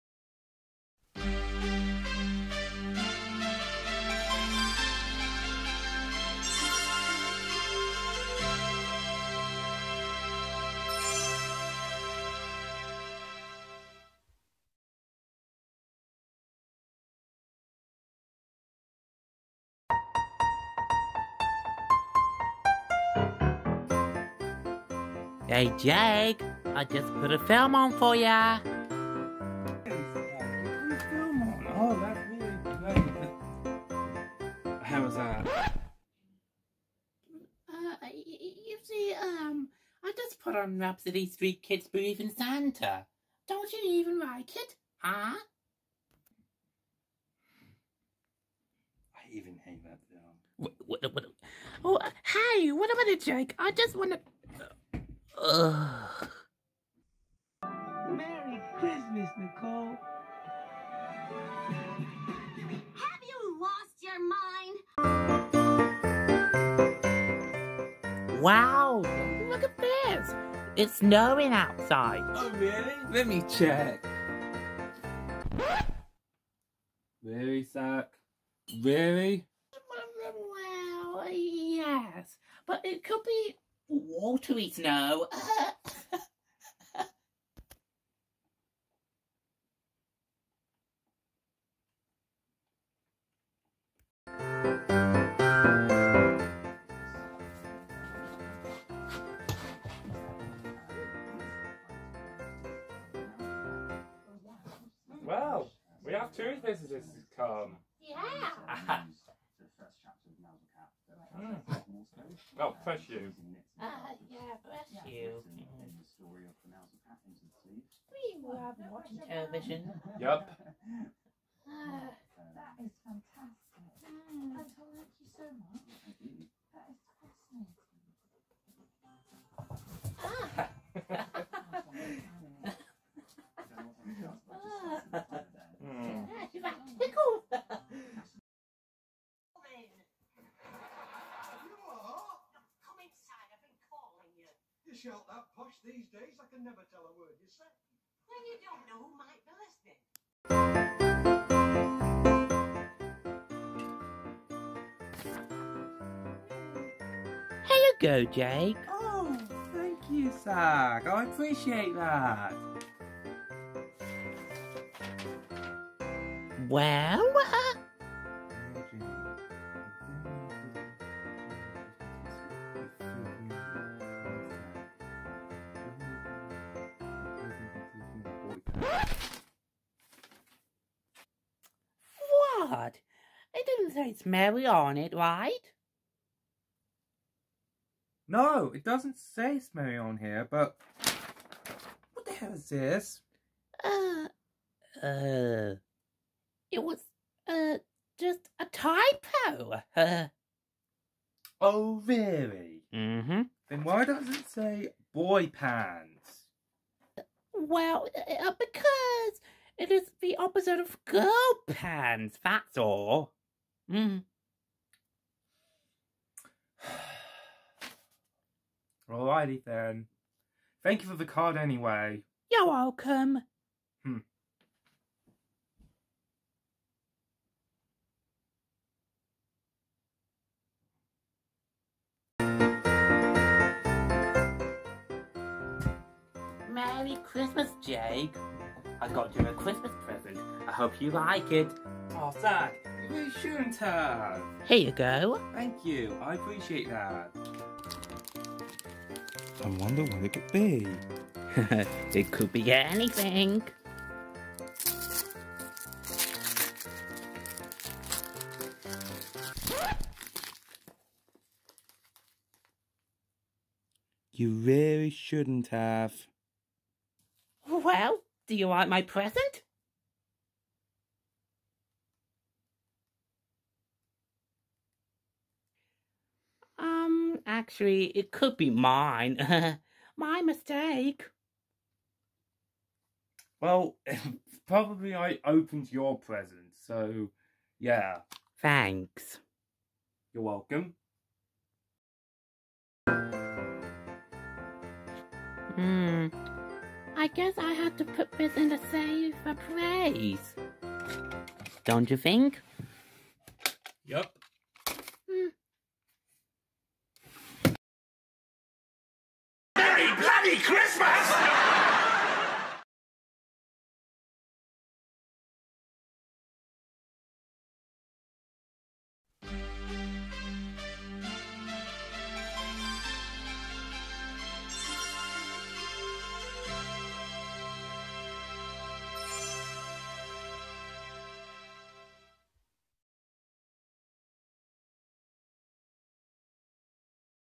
Zackzilla's Christmas Special
Christmas with Zack & Jake

A remake and alternative version of "Matt Ruins Christmas" by @matt